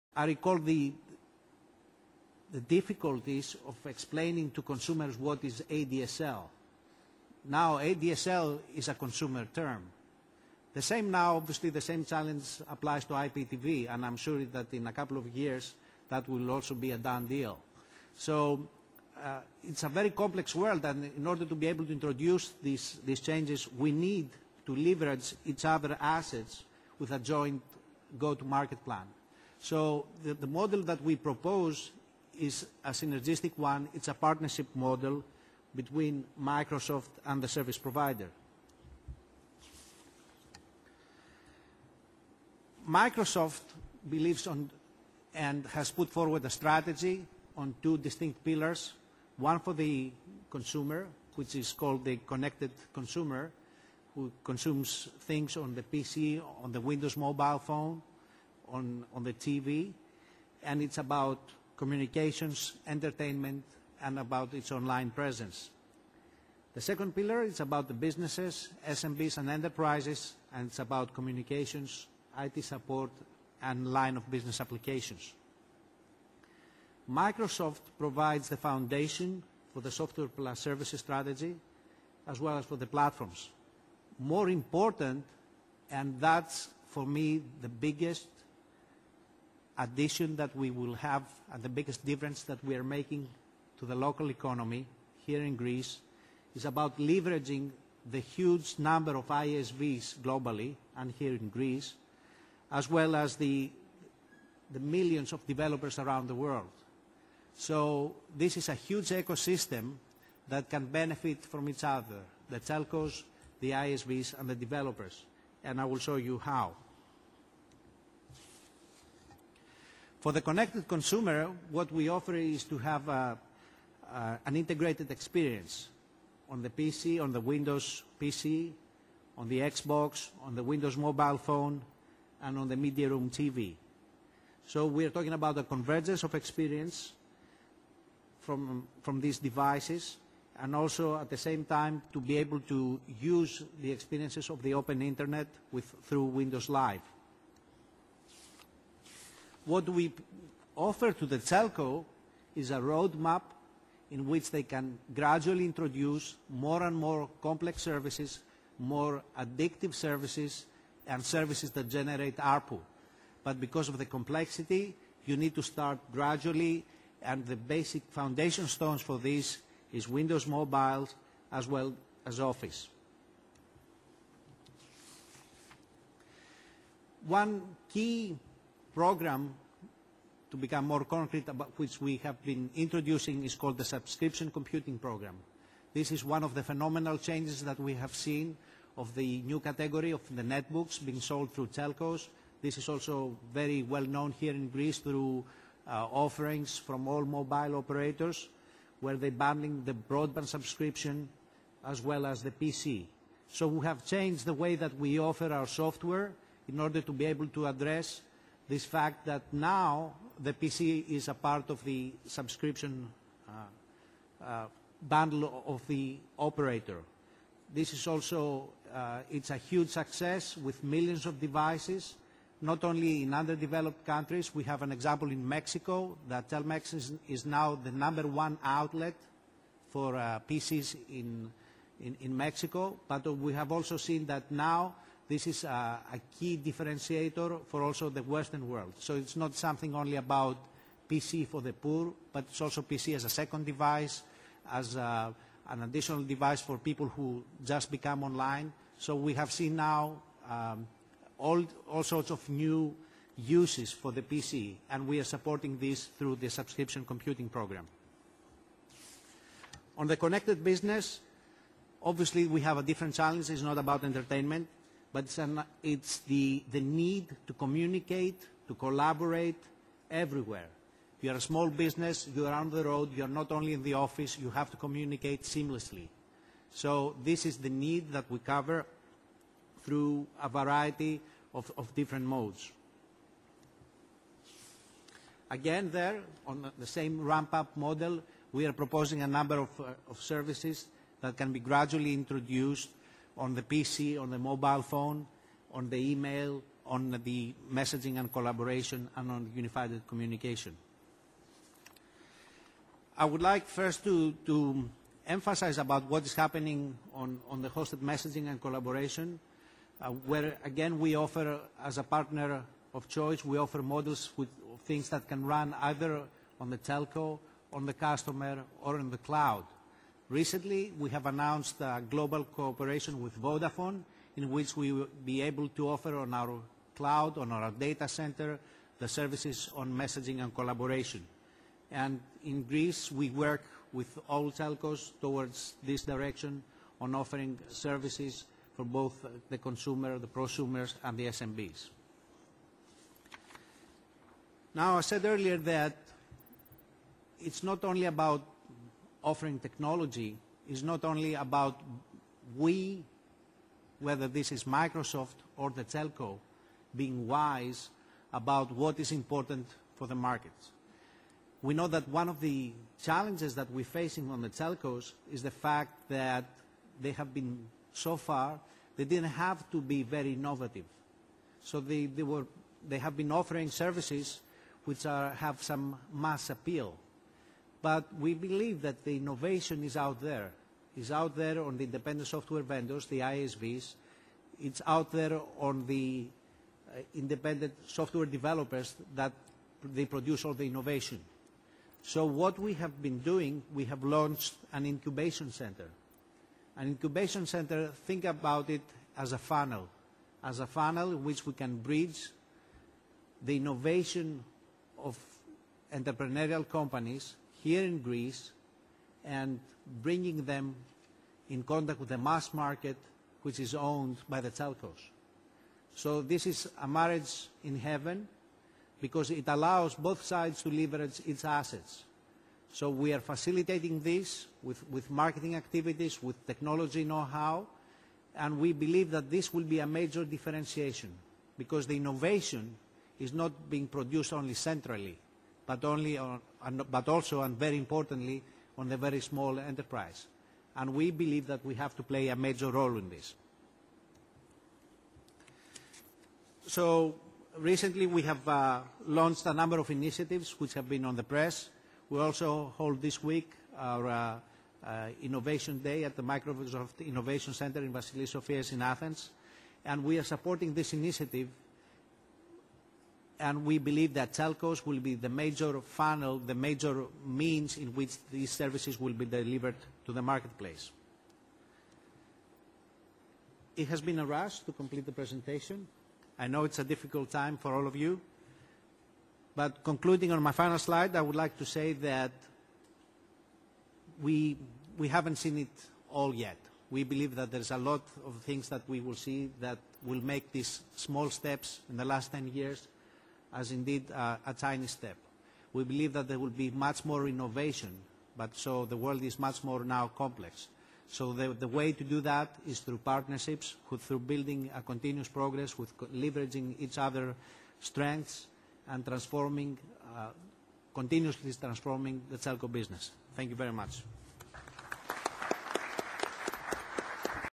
Ομιλία Α. Παπασπυρίδη στο 4ο Διεθνές Συνέδριο
Ομιλία Α. Παπασπυρίδη στο 4ο Διεθνές Συνέδριο για την Ευρυζωνικότητα (Μέρος 2ο)